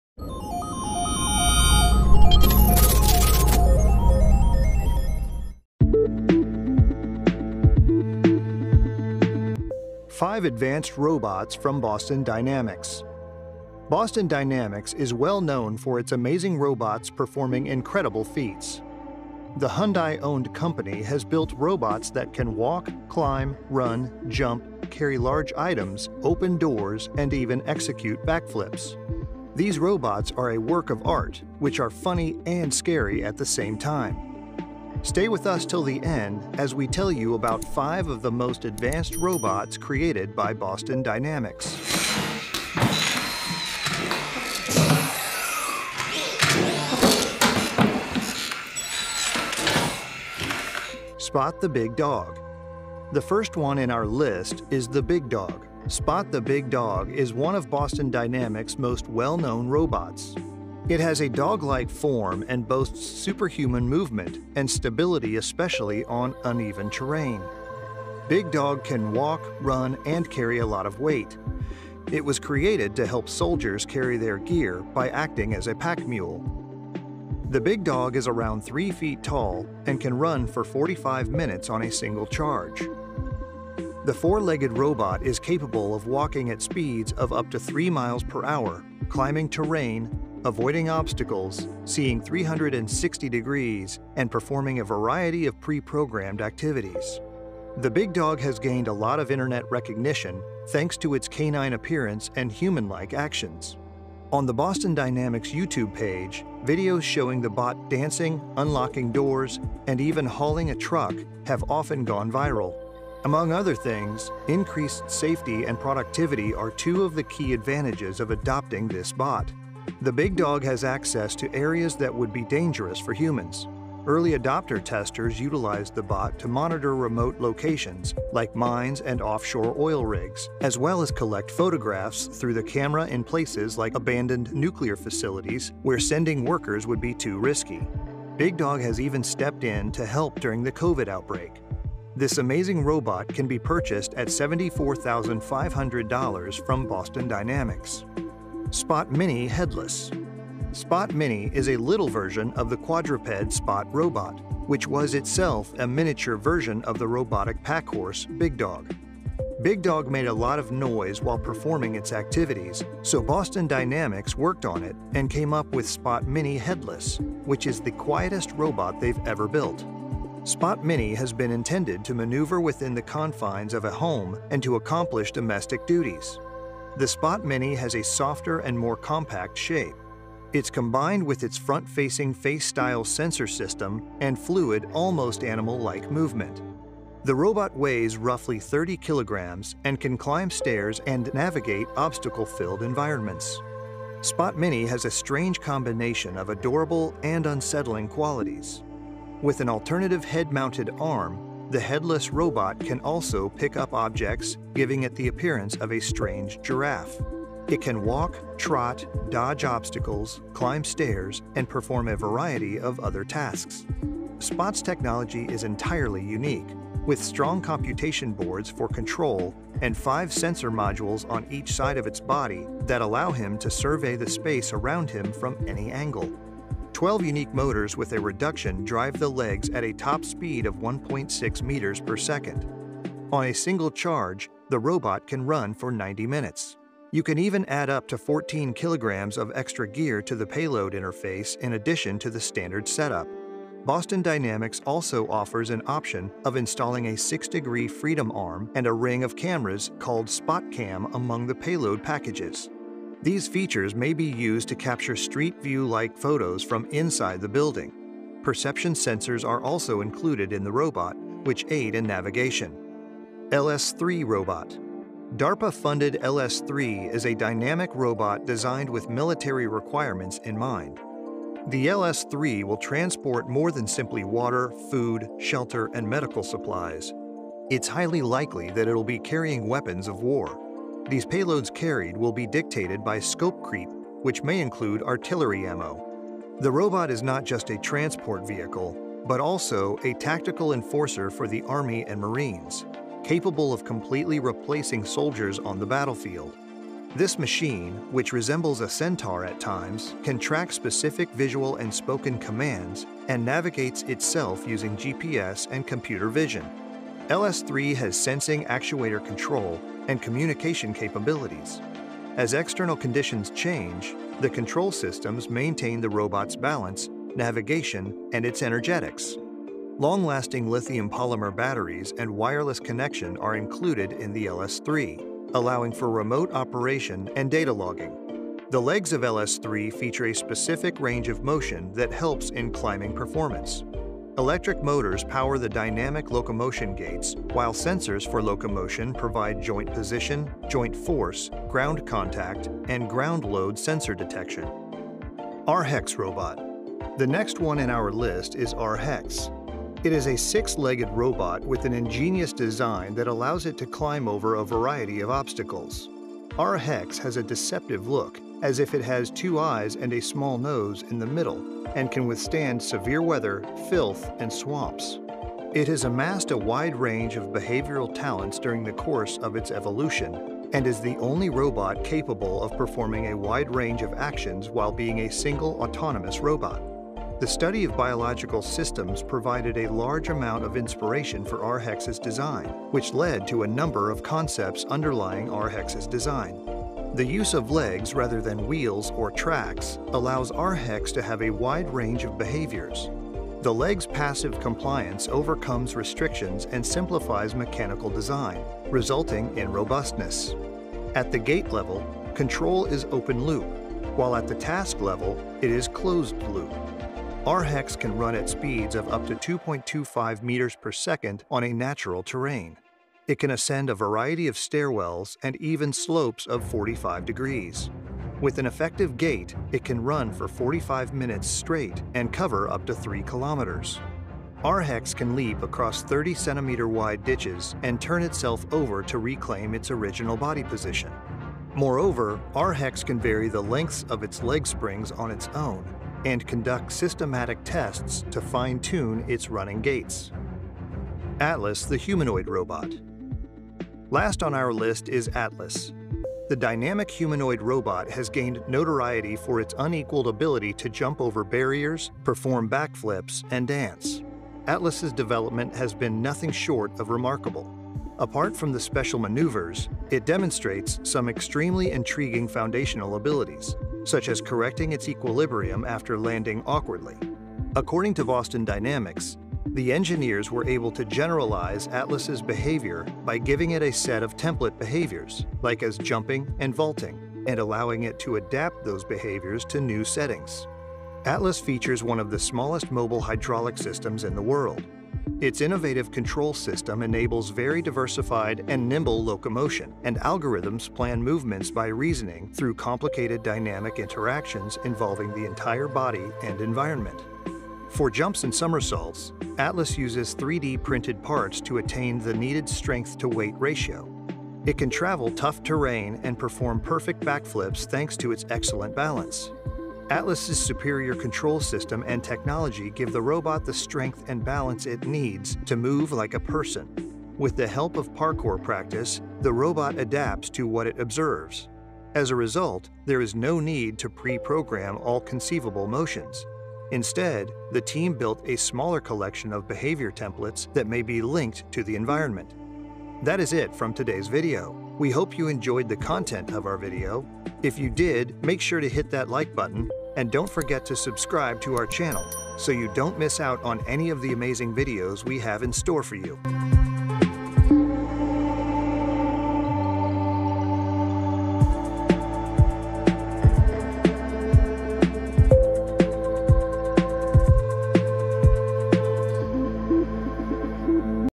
Robotics: These robots can make SOLDIERS Obsolete | Futuristic
Boston Dynamics is a big name in the market of robotics. In this video, we'll show you how a Boston dynamic robot performs actions better than humans. whether it is the Boston dynamics atlas or they're Boston dynamics military robots, you'll surely enjoy their activities and will confess why we say that these Boston Dynamics robots can even make soldiers obsolete.